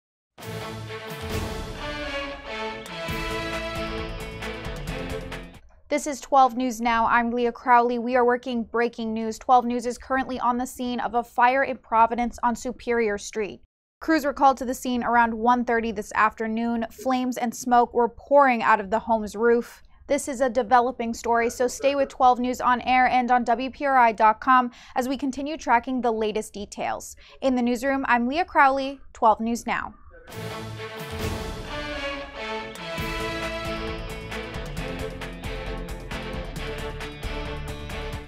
12 NEWS NOW: Crews battling fire at Providence home
First responders are at the scene of a house fire in Providence’s West End.